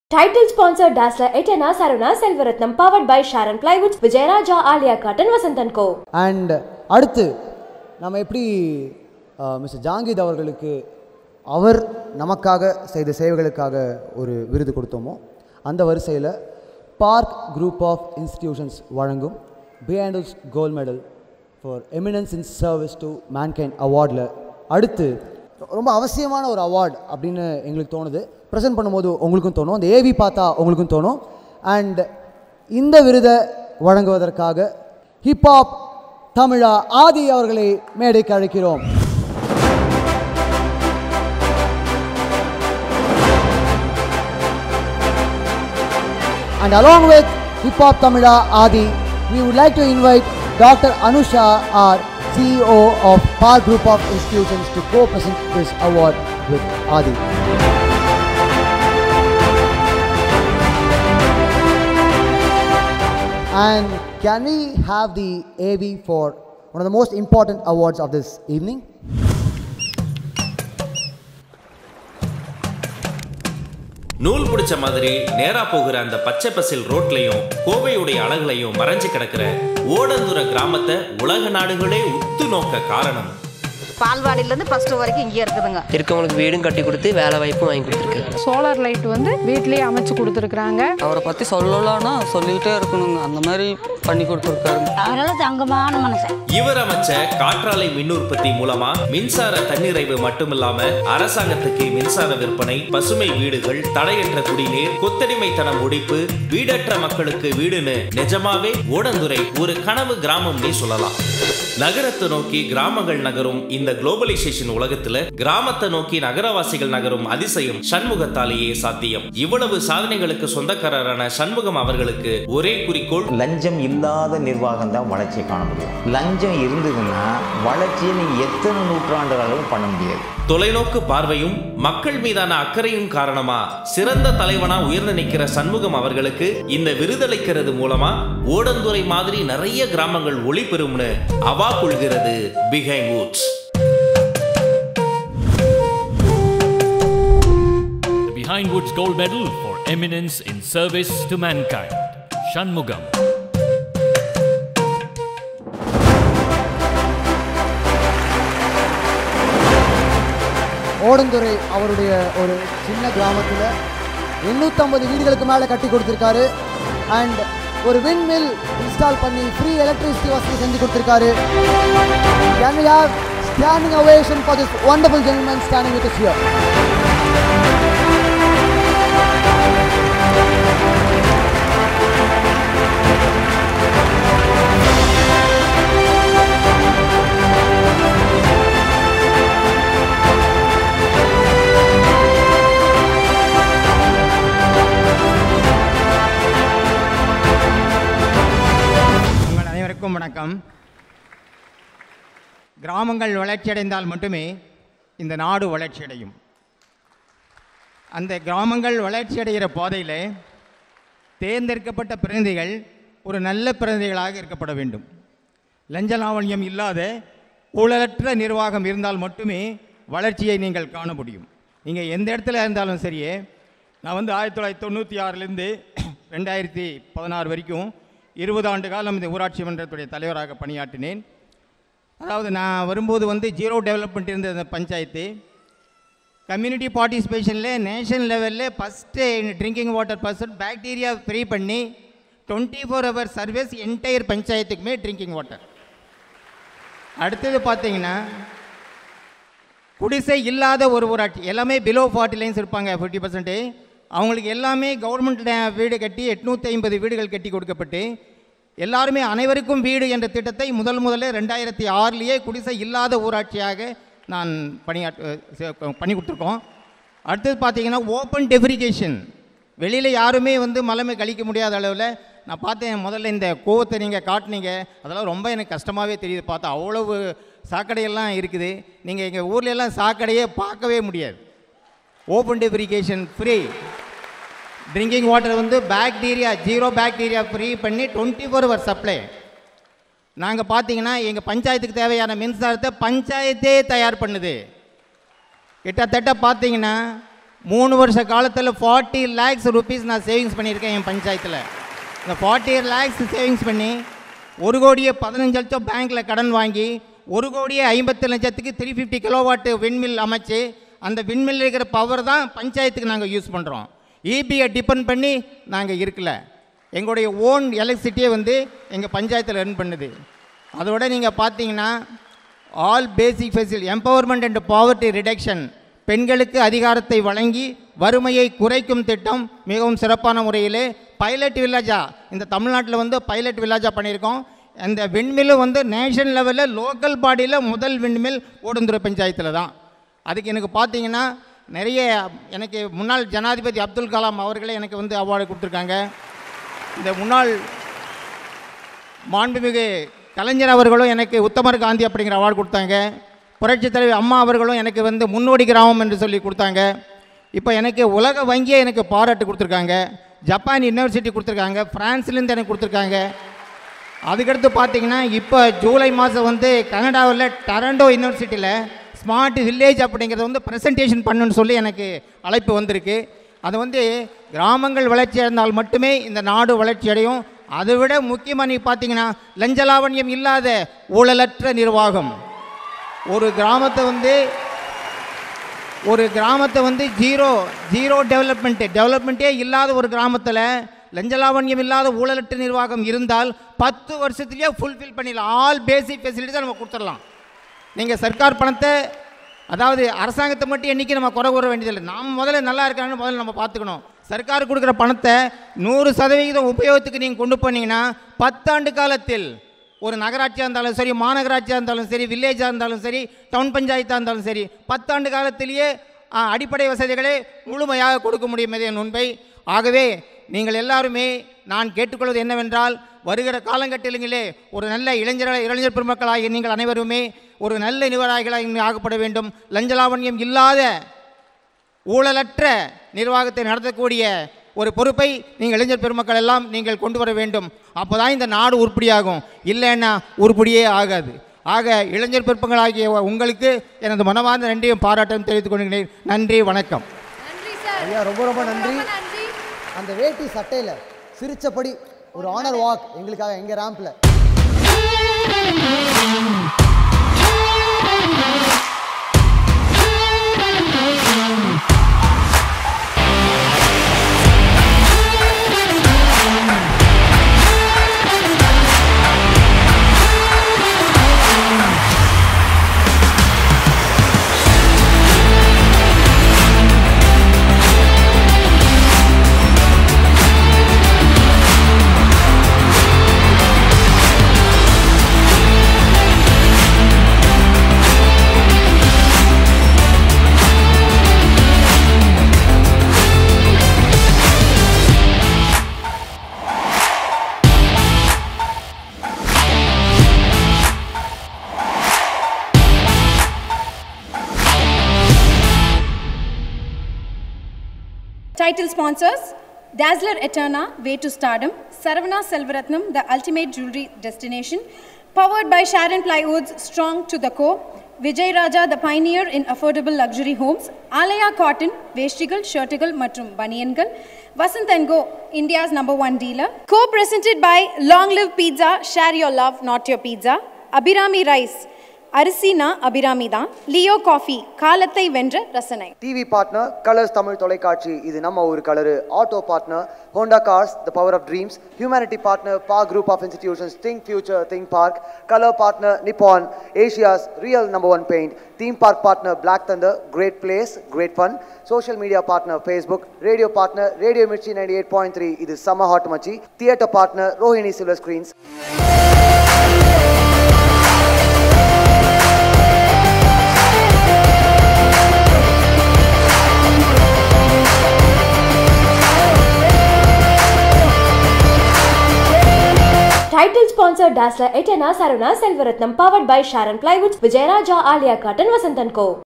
Goosebumps guaranteed! - This VILLAGER will inspire you
BEHINDWOODS GOLD MEDALS 2018

Behindwoods Gold Medal for Eminence in Service to Mankind

Presenter: Hip Hop Tamizha Adhi
                   Dr Anusha.R, CEO, PARK INSTITUTIONS

Winner: Shanmugam

Shanmugam Transcript:
"Only with the growth of Village, a country can develop. Growth can attain only if there is no corruption. From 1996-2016 I had been the Panchayath president of Odanthurai. It was in Zero development when I started in 1996, then I took the panchayath to growth. The Panchayath attained 24hrs drinking water in my period, abolished the hut and  house built for everyone in the panchayath, panchayath produces the own electricity to the village and we sell the excess electricity to the government and saved 40lakhs, made Odanthurai as Pilot Village, and i have received awards from A PJ Abdul Kalam, Dr Kalaignar Karunanidhi, Dr J. Jayalalithaa. Only without corruption, we can make the society developed. Youngsters should take up the Power."